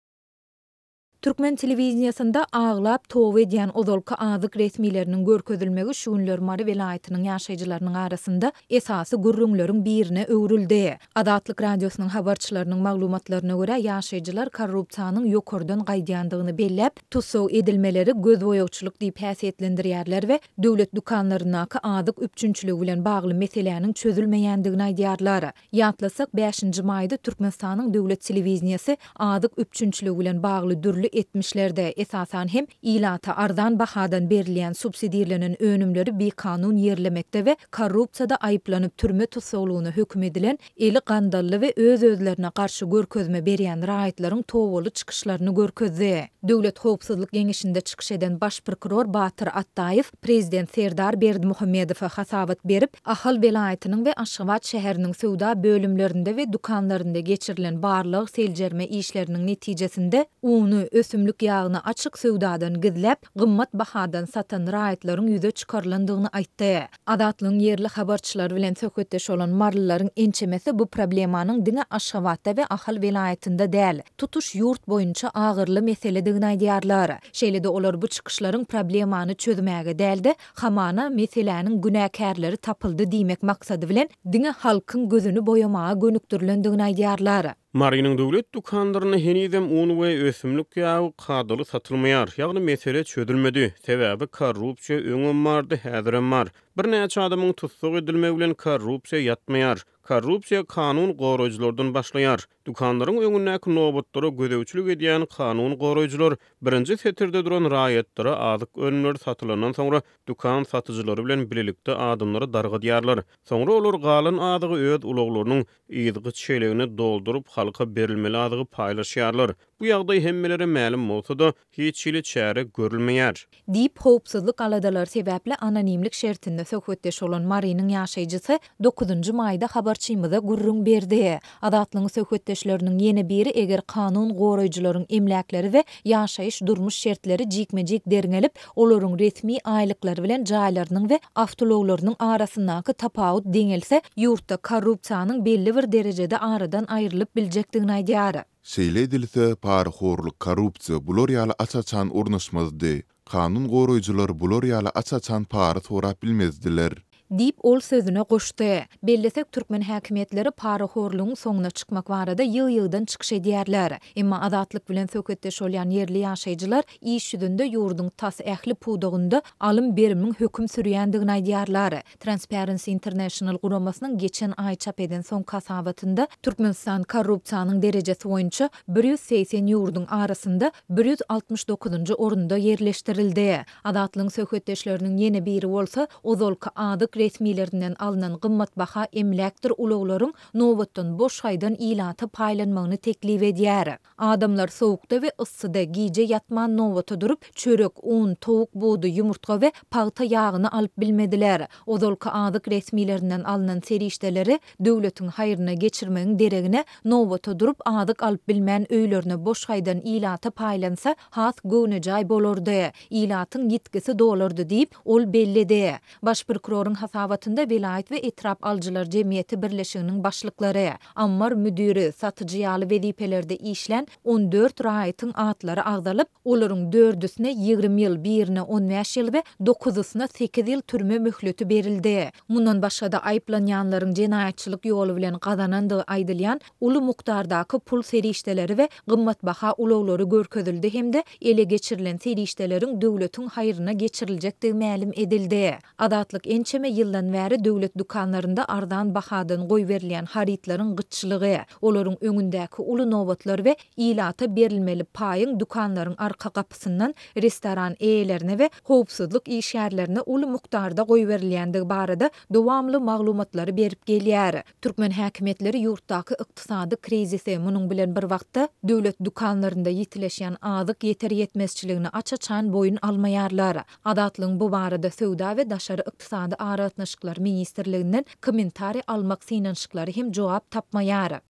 Marylylar ozalky azyk resmileriniň tussag edilmegini "gözboýagçylyk" häsiýetlendirýär
Türkmen telewideniýesinde aglap "toba edýän" ozalky azyk resmileriniň görkezilmegi şu günler Mary welaýatynyň ýaşaýjylarynyň arasynda esasy gürrüňleriň birine öwrüldi. Azatlyk Radiosynyň habarçylarynyň maglumatlaryna görä, ýaşaýjylar korrupsiýanyň ýokardan gaýdýandygyny belläp, tussag edilmeleri "gözboýagçylyk" diýip häsiýetlendirýärler we döwlet dükanlaryndaky azyk üpjünçiligi bilen bagly meseläniň çözülmeýändigini aýdýarlar.